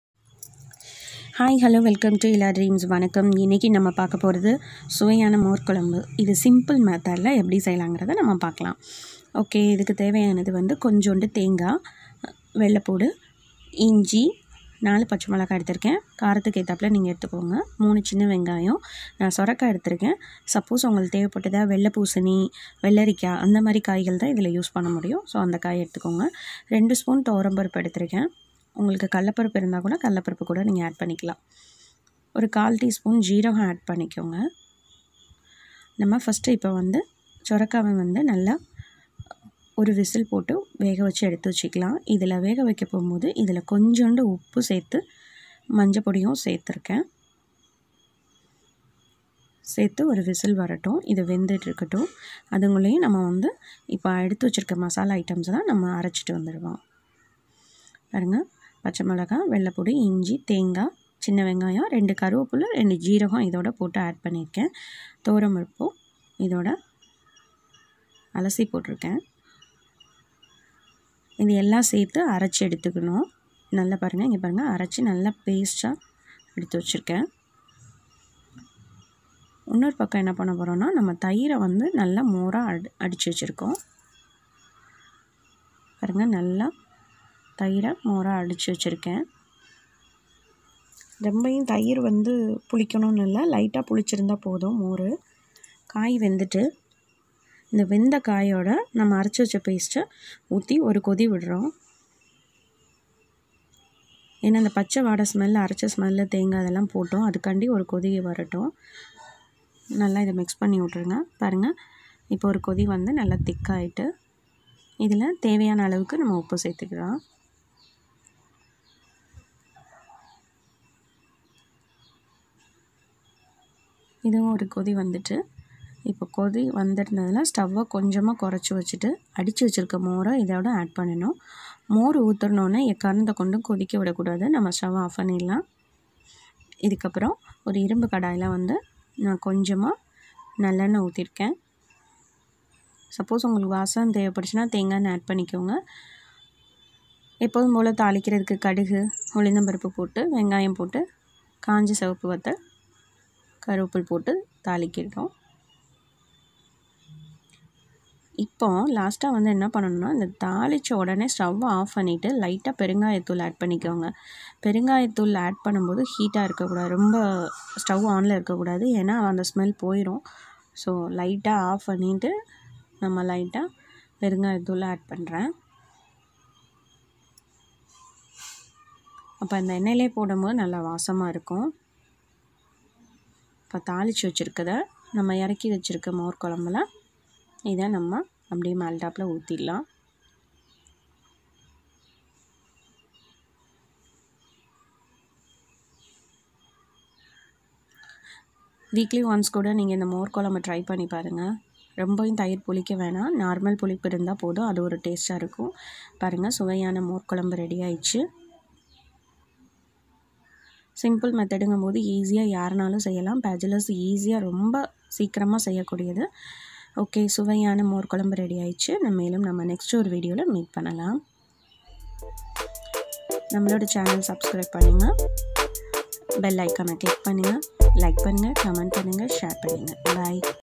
சுலபமான முறையில் மோர் குழம்பு செய்யலாம்/Easy curry/Kerala style moru curry recipe in tamil
Coconut
Jeergam
Onion
Chilli
Garlic
Ginger
Salt
Bottle gourd